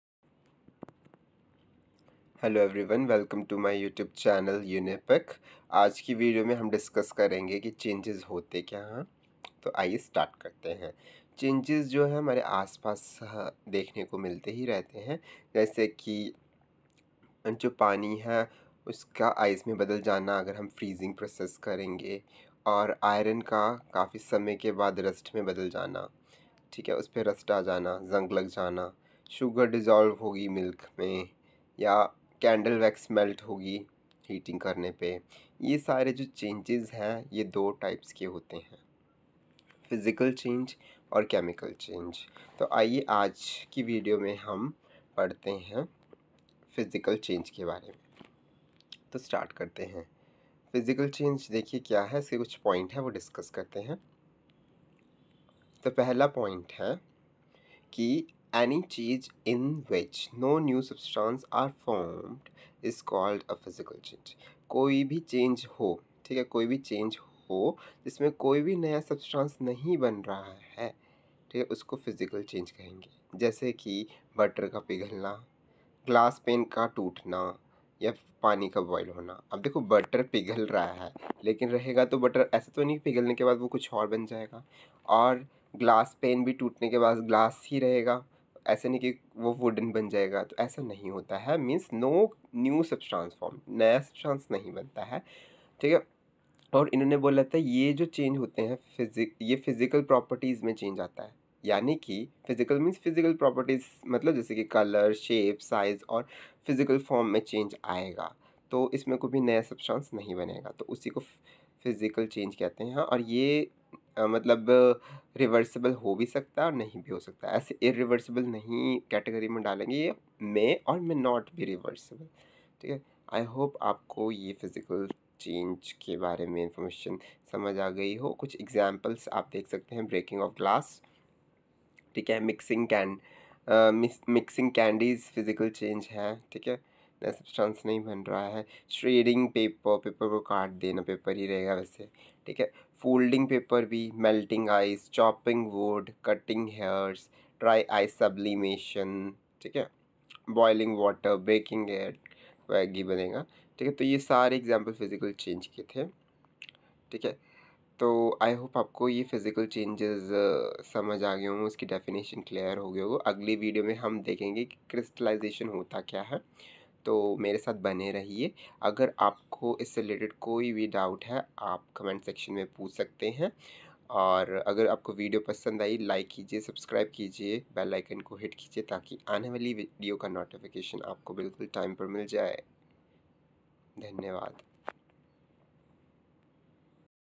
Science || What is change? || Types of change || What is physical change? || With examples...😔👍
Physical Change

When matter changes its observable properties, we can say it has undergone a physical change.

Physical change is a type of change where the physical properties of matter change. A change of state of matter, change in colour, odour, solubility, etc. all are examples of physical change.

During a physical change, neither the composition nor the chemical nature of matter is changed.  During this change, molecules rearrange themselves without affecting the internal composition. A physical change doesn’t affect the chemical property.